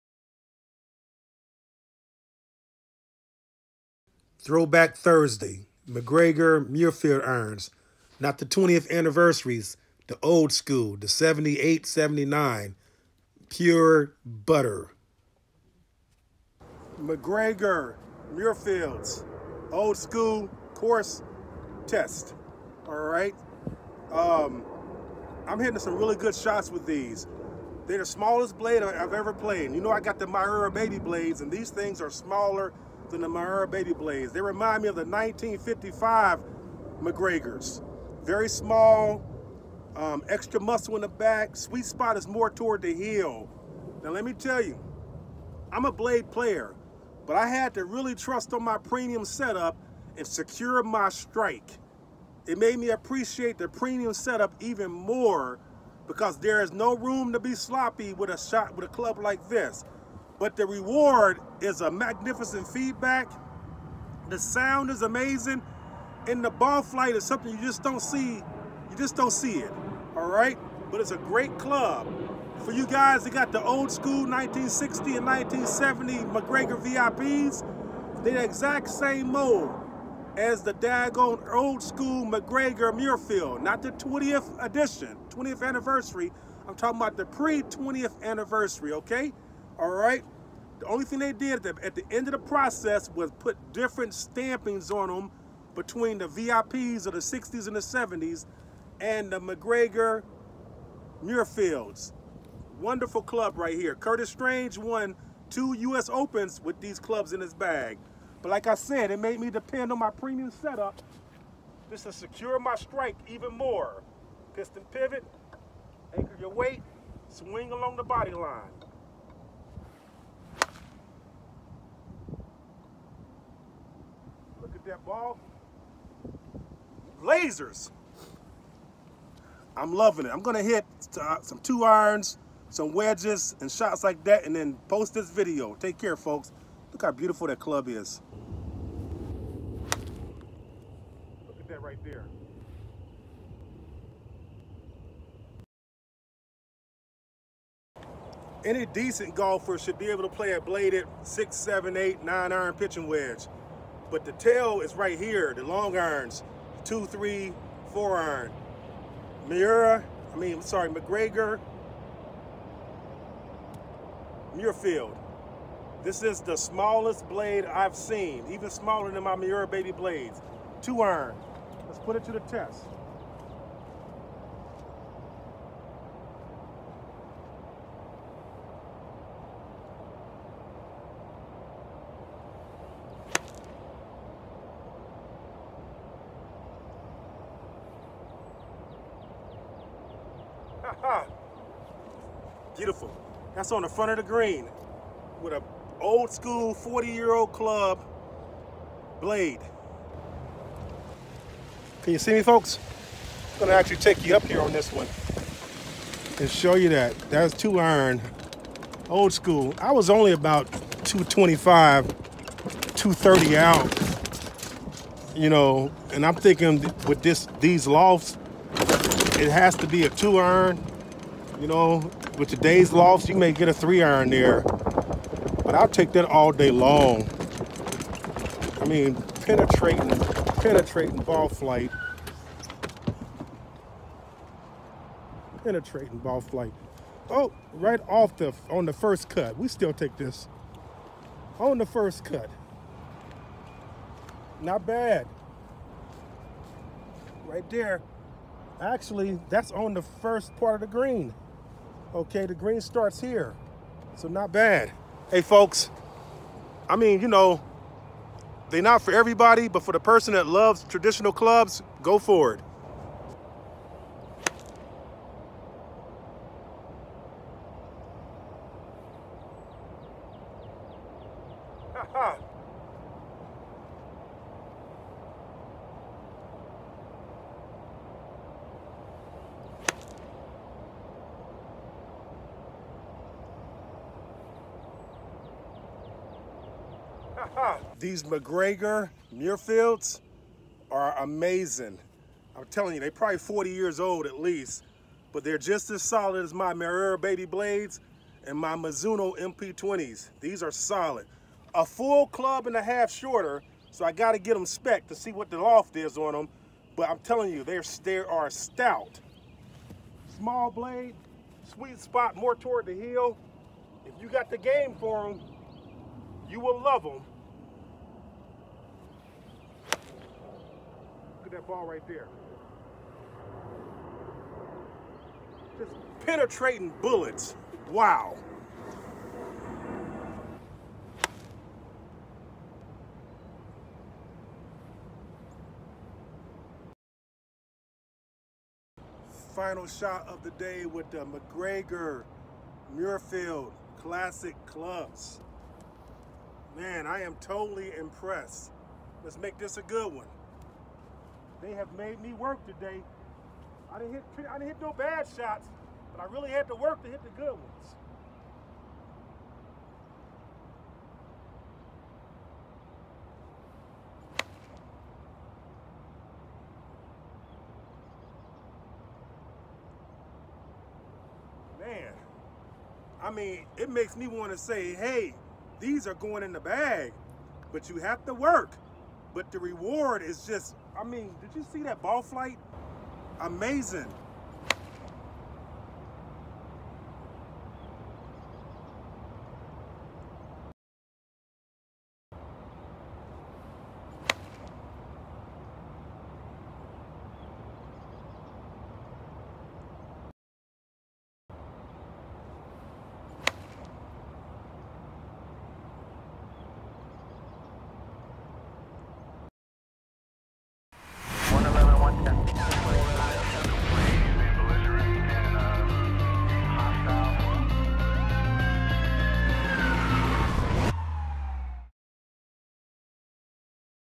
Throwback Thursday MacGregor Muirfield Blades
A round a golf with 50 year old clubs do yoo think you can still make pars and play a decent round ? I think you can if you make the correct adjustments with distance control. Remember the older models are most likely 4-7 degress weaker in loft then your current clubs. But i have to admit the MacGregor Muirfield Blades are one of the most demanding blades I have ever played.